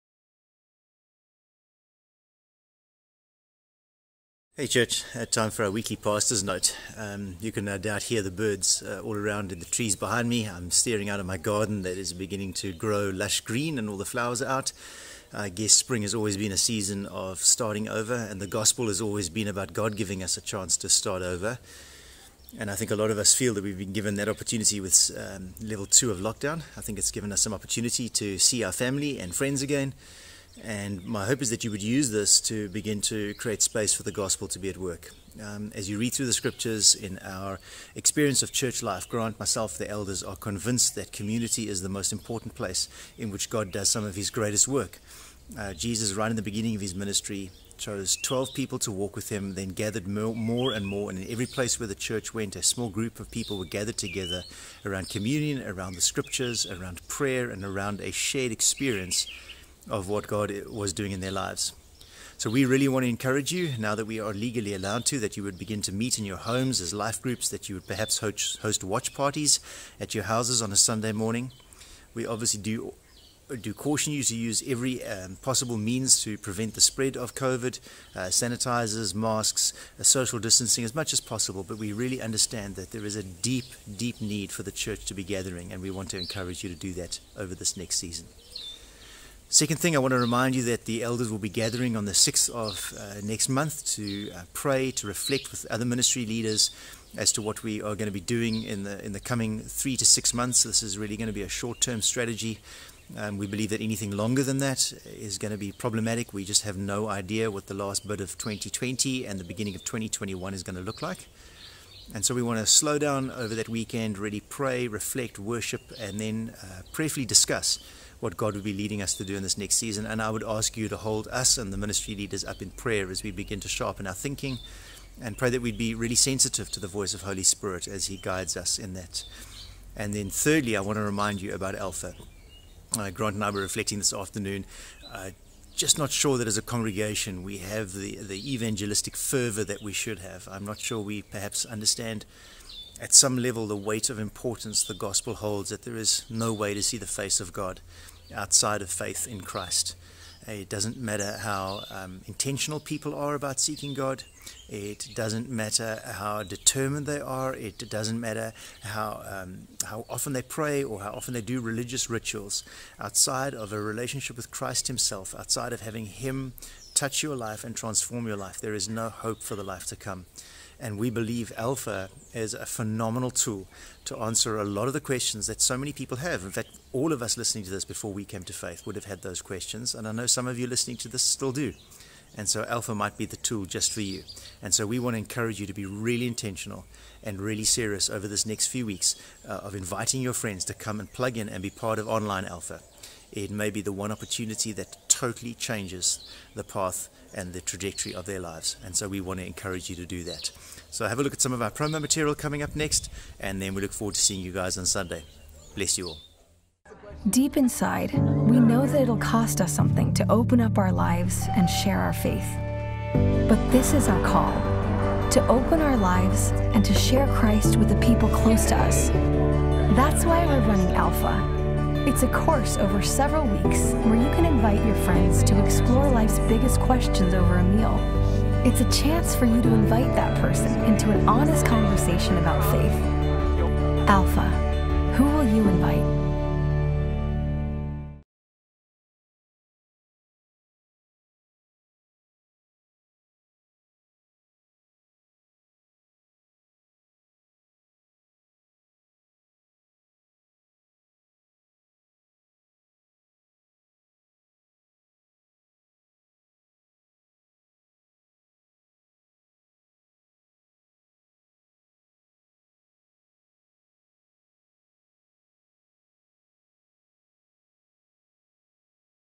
Pastor's Note (26 August 2020)
In this week's Pastor's Note, Brent reminds us that within community is where God does some of his greatest work. There is a deep need for the church to gather and we are encouraged to use lockdown level 2 to engage responsibly with friends, family and our LifeGroups.

Our upcoming Alpha Online course starts on Tuesday, 15 September.